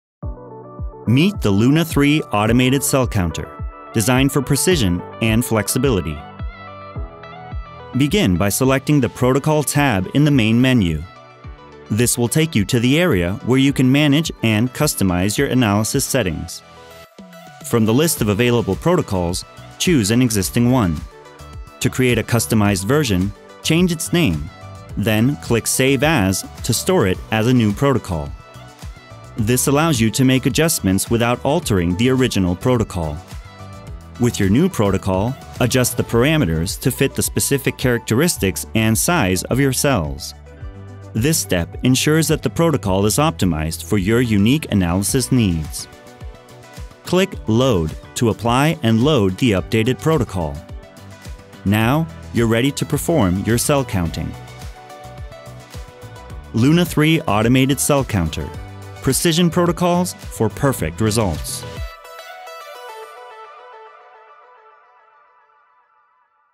How to Customize Protocols
Learn How to customize your Luna-III to bring the best result for your cell.